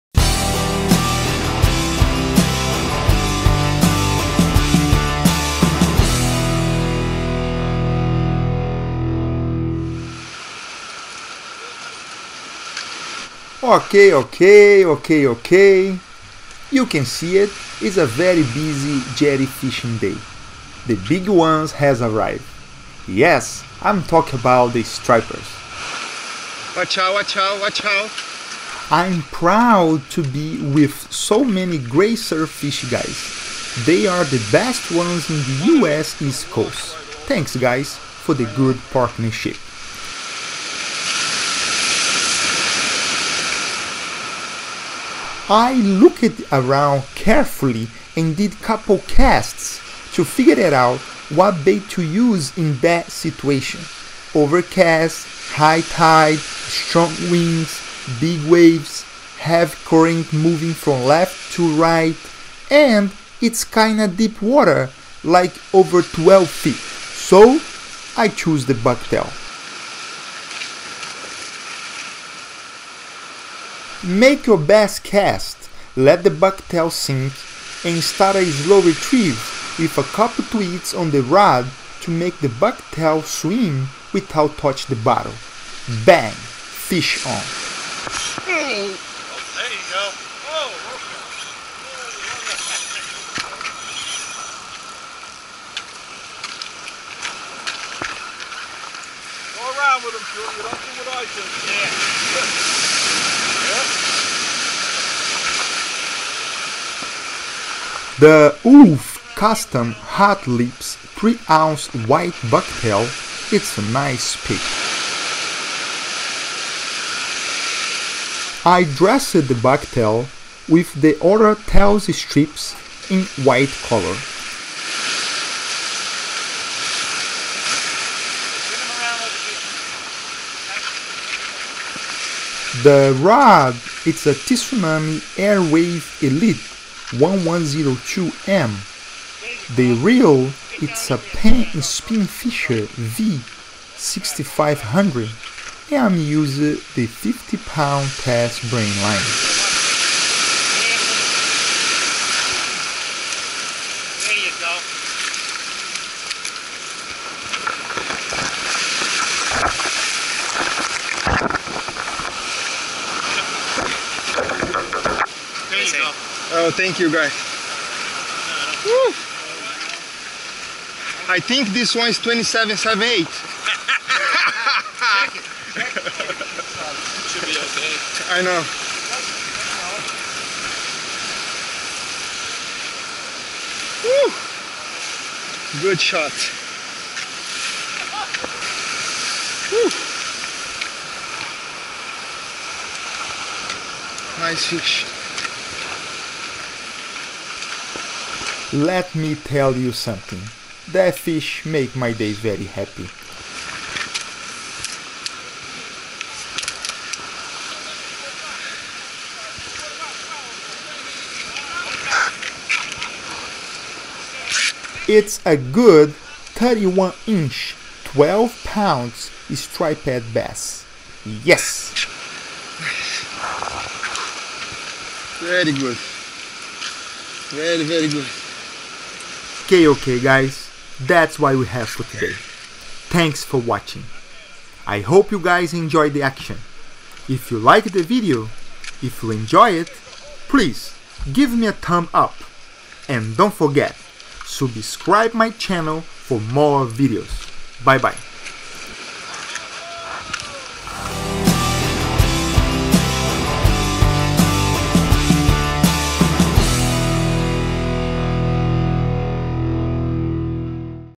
The Huge Striped Bass
It's really busy jetty fishing day. All great fisherman has arrive. Everybody catch a striper. Enjoy the action.